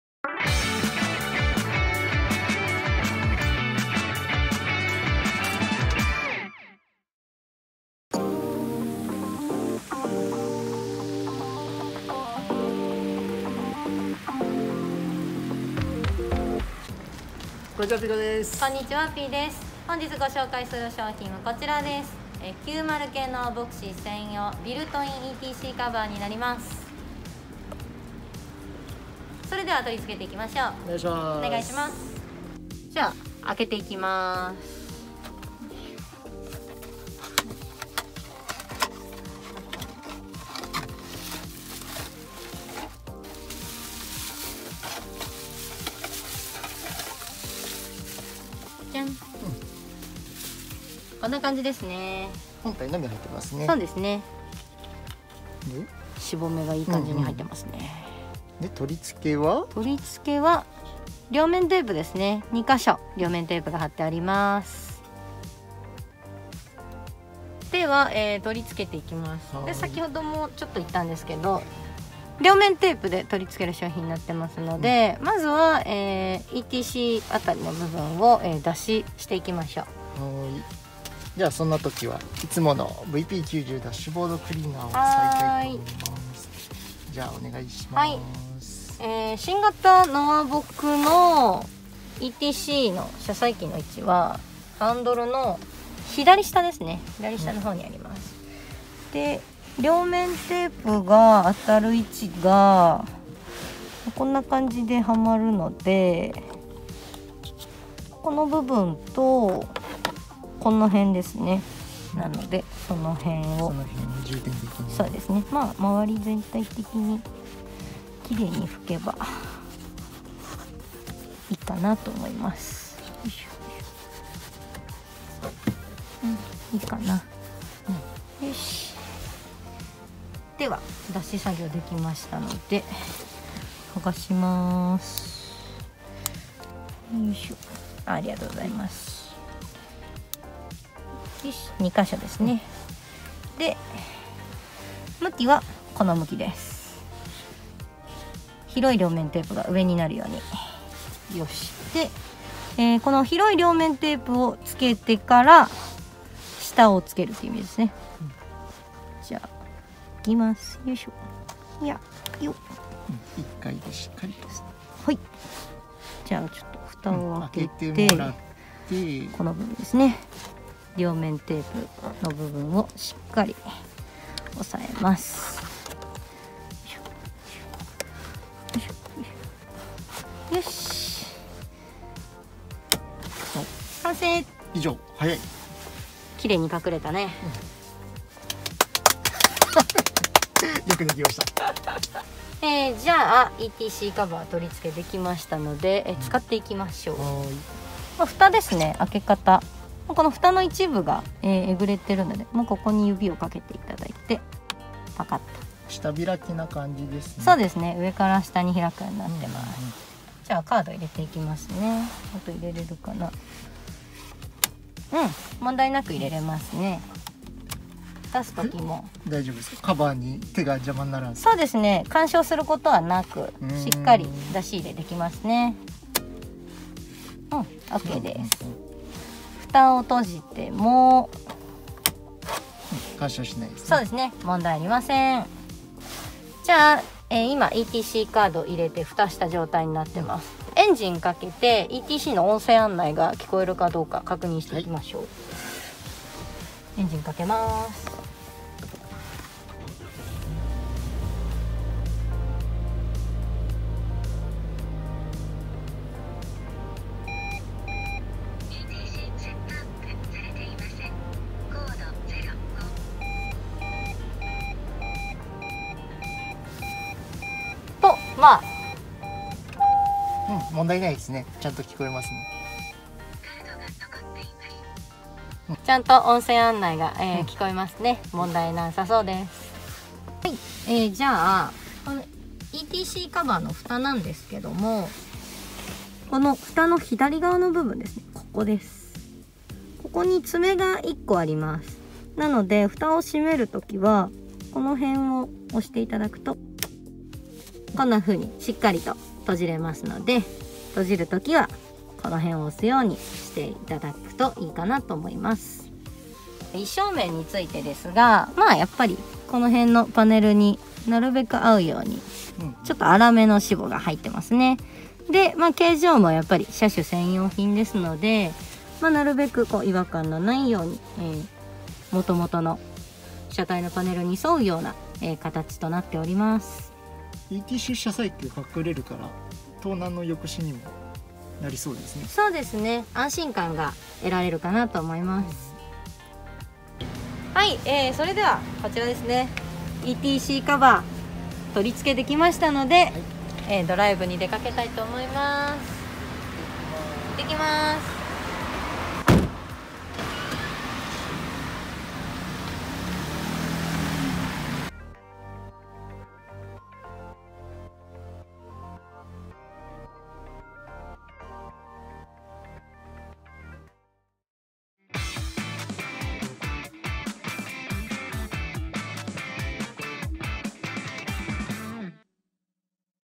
【90系ノア・ヴォクシー専用】ビルトインETCカバー！カード挿入部を隠して見た目すっきり&盗難抑止に！ノア ヴォクシー 90系ヴォクシー
90系ノア・ヴォクシー専用ETCカバーの紹介になります。カード部を隠して外から見えなくすることができます。取付は両面テープで貼り付けるだけでとても簡単です。槌屋ヤック 車種専用品になります。

現在弊社PAYPAYモールにて販売もしております！

槌屋ヤックのオフィシャルYouTubeチャネルです。